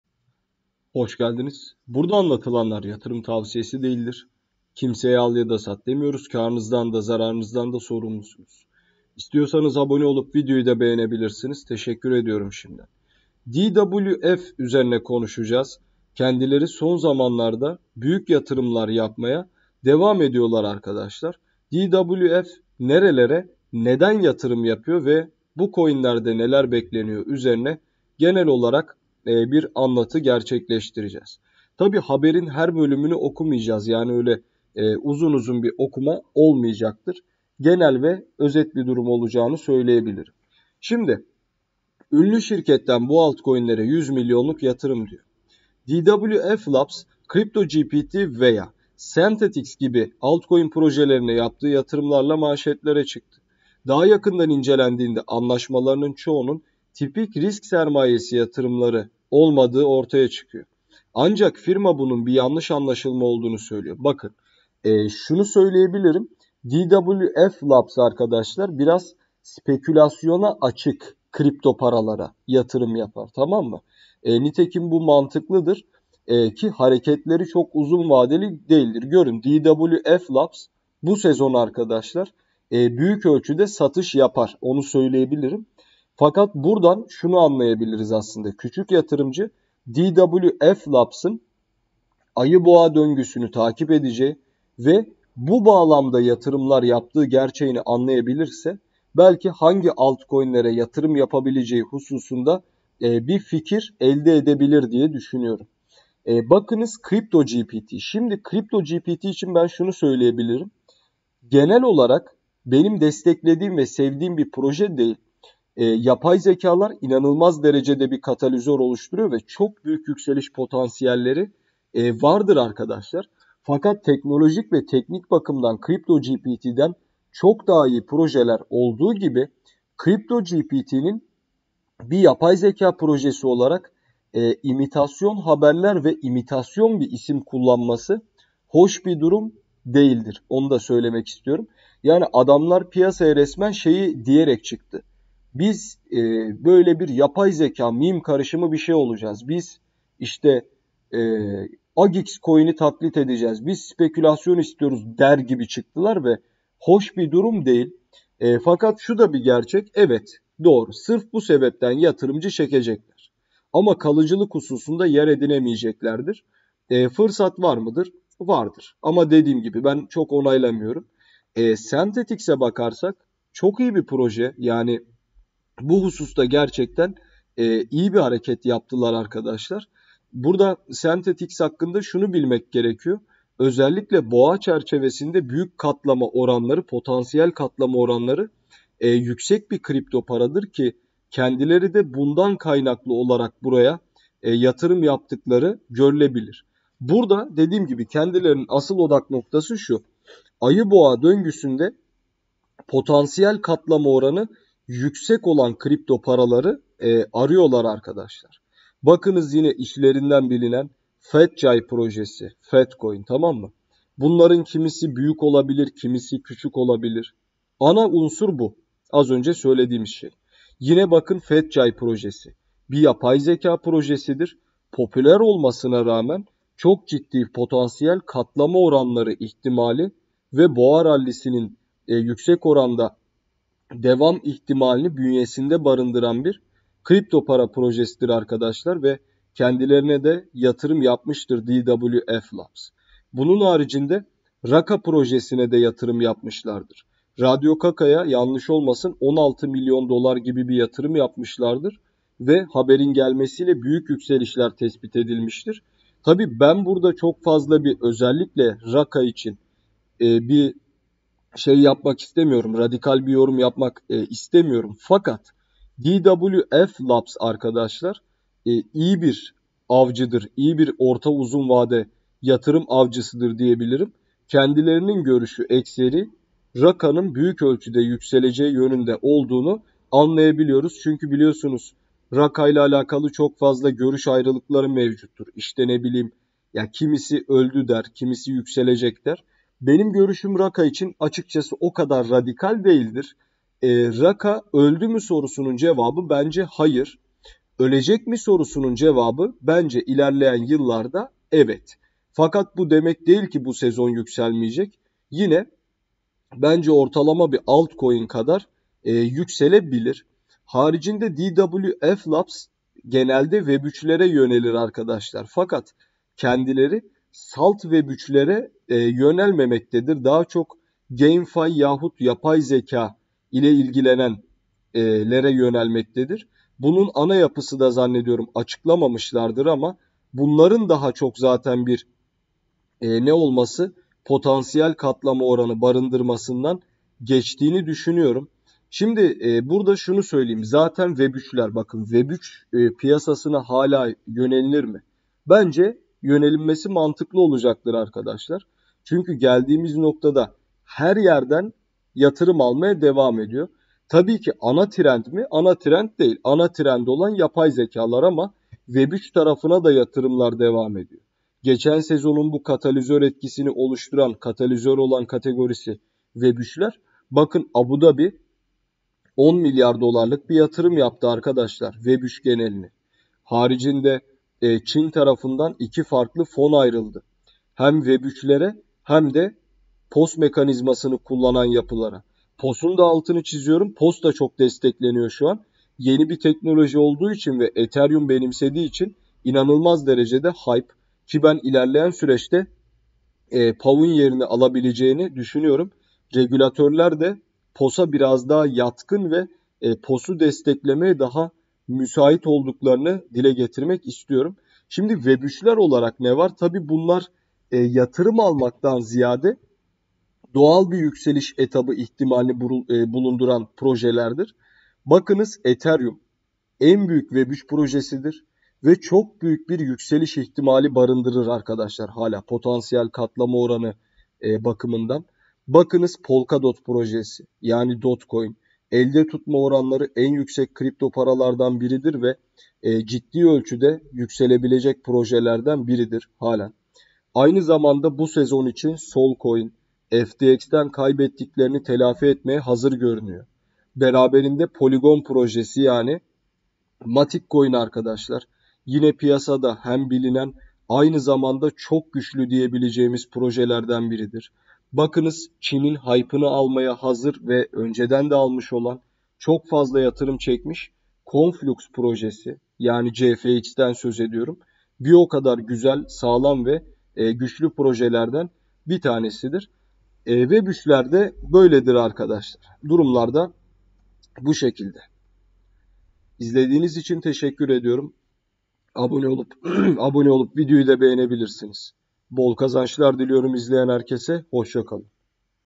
SON DAKİKA: RACA COİN, ALDIĞI MİLYONLARCA DOLARLIK YATIRIMLA 15X RALLİSİ BAŞLATTI!🚀RADİO CACA PUMP!🔥
Bu videomuzda kripto borsalarında oldukça popüler bir coin olan RACA coin'i (Radio Caca) inceledik. Raca coin'in destek ve direnç noktalarını belirterek kısa, orta ve uzun vade hedeflerinden söz ettik. Raca alım ve satım noktalarını belirleyerek, hangi noktadan almalıyız? Gibi sorulara yanıt olmaya çalıştık. Raca, uzun vadede yakaladığı popülerite ve beklenen hacimle Binance'ın da olumlu yaptırımlarıyla 2022'de çok büyük yükselişler yapabilir. Raca, yeniden yukarı sıçramak üzere MA50'ye doğru ilerliyor. Raca son yaşanan gelişmelerle birlikte 21 Nisan 2022 sabahı saat 8.00'da KuCoin borsasında listelendi. Bu videoda yatırım tavsiyesi verilmemiştir.